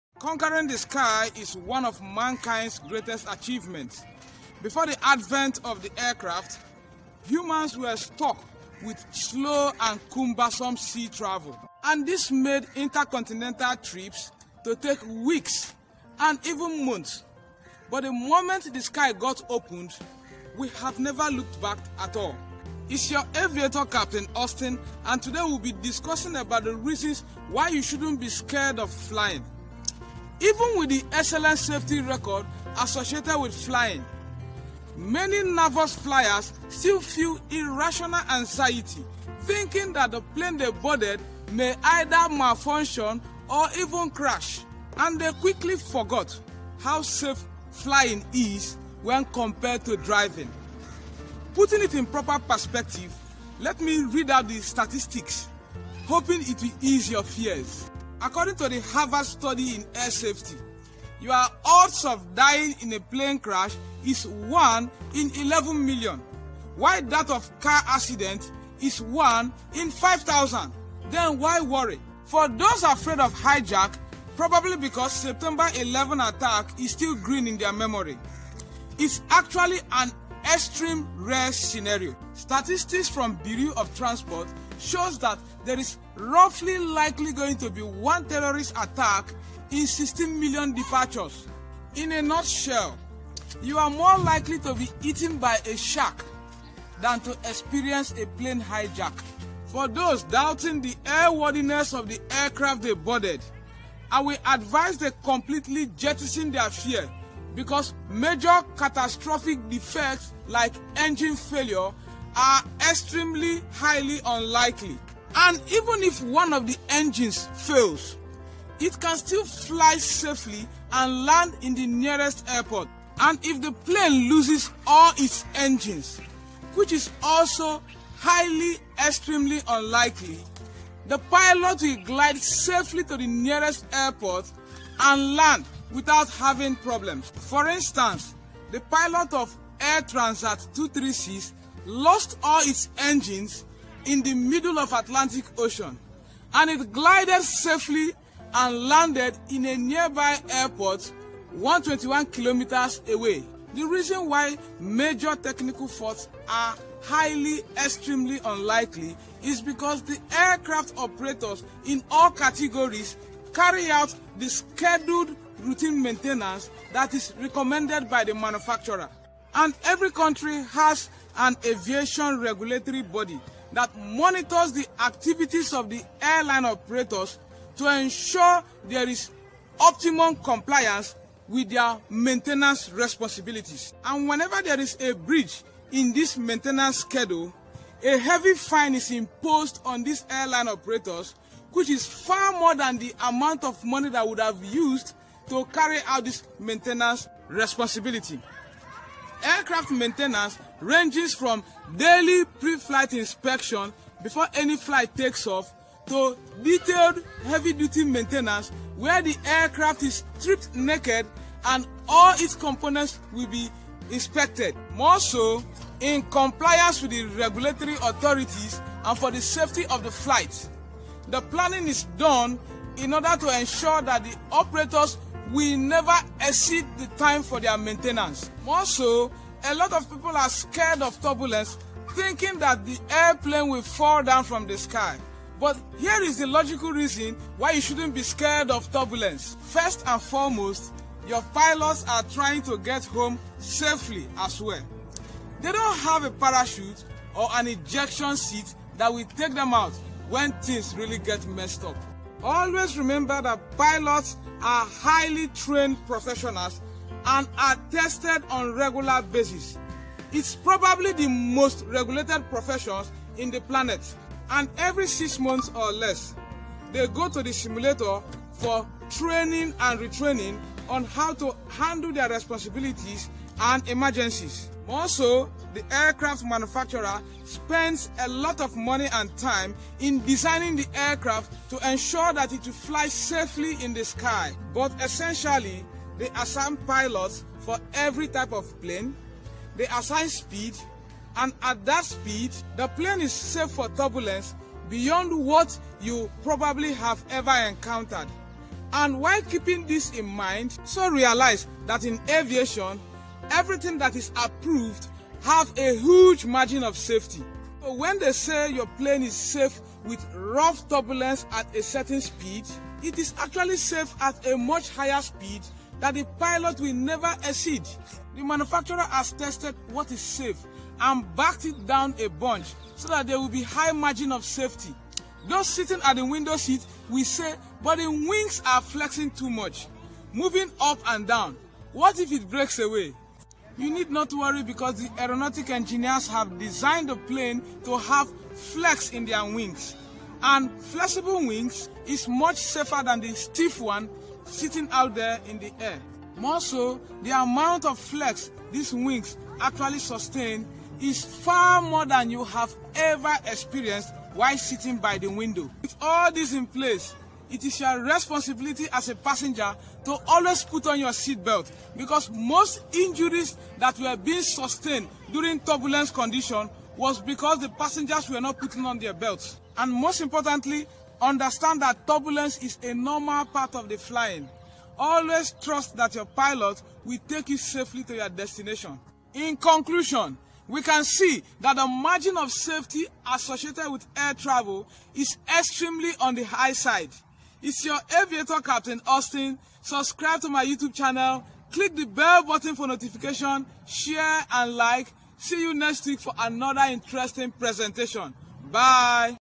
WHY YOU SHOULDN'T BE SCARED OF FLYING
The reasons why you shouldn't be scared of flying

Conquering the sky is unarguably one of the greatest mankind's achievement. Before the advent of the aircraft, humans were stuck with slow and cumbersome sea travel and this made intercontinental trips to take weeks and some times month, but the moment the sky got open, we haven't looked back at all.

However, many narvous flyers still feel irrational anxiety that the plane they boarded may malfunction and crash due to a technical failure that may develop in the course of the journey. But in real sense, major technical failure is almost not going to be apparent because the aircraft undergoes detailed scheduled compulsory maintenance programe from time to time and there are regulatory agencies that ensure the airline operators give hundred percent compliance with their maintenance responsibilities.
Starting from daily routine preflight inspection before any flight takes off to detailed heavy duty maintenance checks where the aircraft is stripped naked and all components are carefully well inspected  to ensure it stays airworthy through out it's service time.

Furthermore, some persons are afraid of boarding a plane that will be hyjacked. This is another highly unlikely event. According to the Beaurea of transportation, there is likely going to be one terrorist attack in 16 million departures. In a nut shell, you are more likely to be eaten by a shark than to be hyjacked in a plane.

Moreso, a lot of people are scared of turbulence and think it will bring down the plane they boarded, but in actual sense, the aircraft manufacturers had already designed the plane to withstand even the severest of all situations and also included a huge margin of safety. As such,
you need not fear about the pilots inability to control the turbulent condition because they are highly trained professionals and are retrained and tested  every six months in a simulator on regular basis. It's probably the most regulated profession in the world.

In conclusion, air travel remains the safest in the world and be rest assured that the plane you boarded will not crash because it was designed not to crash and every measure to the thinest cue have been put in place to guarantee continued airworthiness always.

TIME STAMPS

Introduction. 0:30

Safety comparison between flying a plane and driving.   1:10

Air safety and hijack   1:39

How proper maintenance prevents air crash 3:48

Why turbulence can't cause a crash 5:25

Importance of flex wing
5:55

Conclusion.  6:39